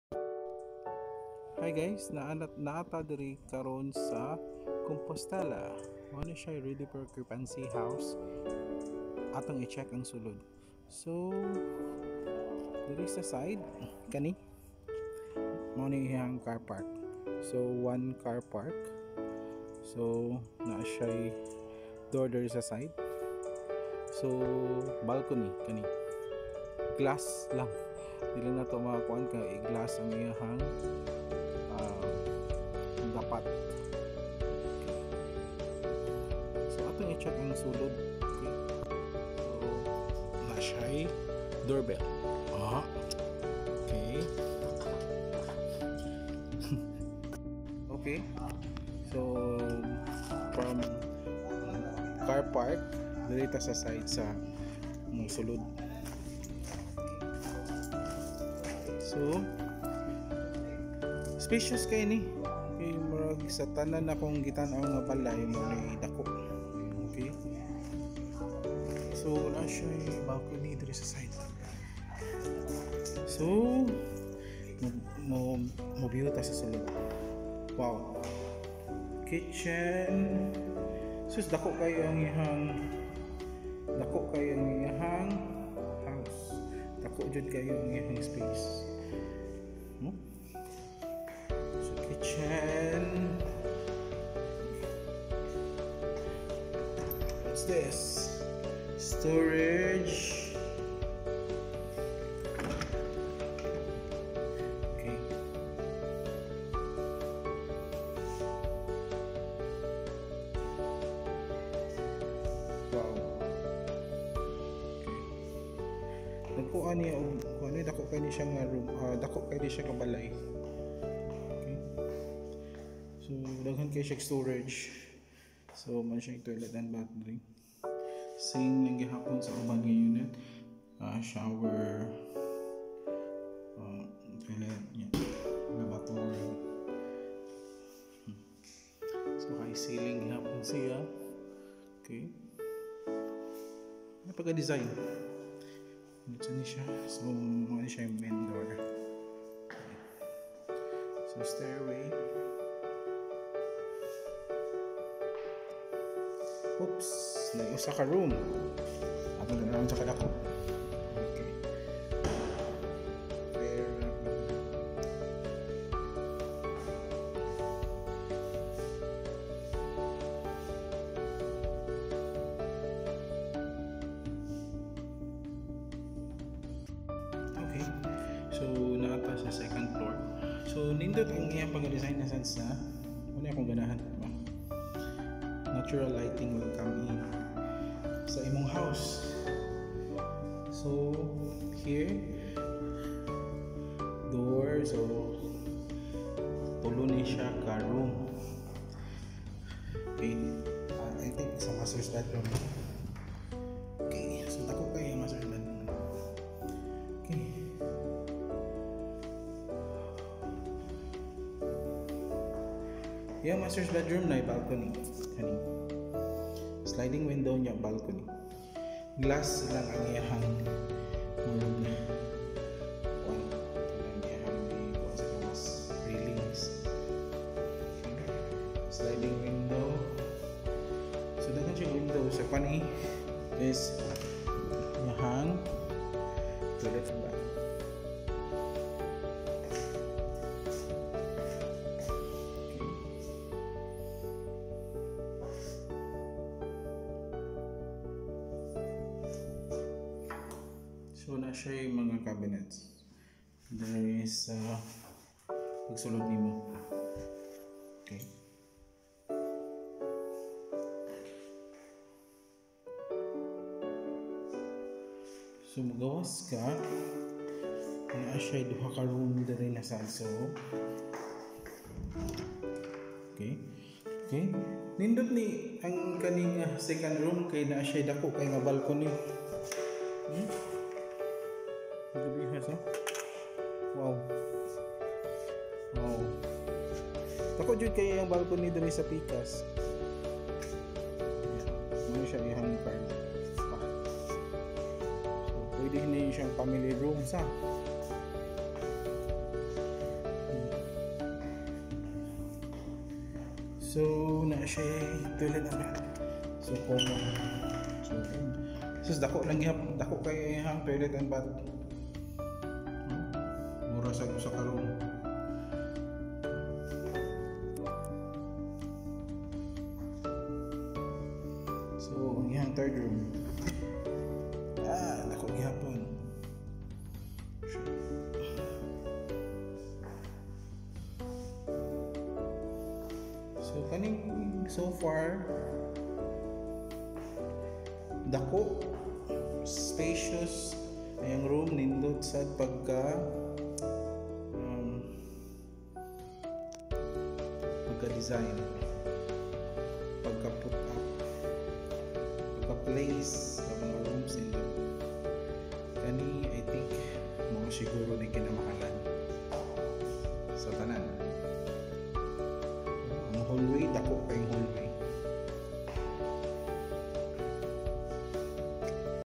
Montana Model Unit Compostela Cebu House
House tour in the beautiful town of Compostela Cebu.

Subdivision Amenity Entrance Gate with Guard House, Swimming Pool, Clubhouse, Basketball Court and the Amenity is overlooking view of the Camotes Sea.

☑️ MONTANA MODEL HOUSE
Compostela Cebu
Lot Area 150sqm | Floor Area 156sqm
3 Bedrooms | 1 Guest Room | 3 Baths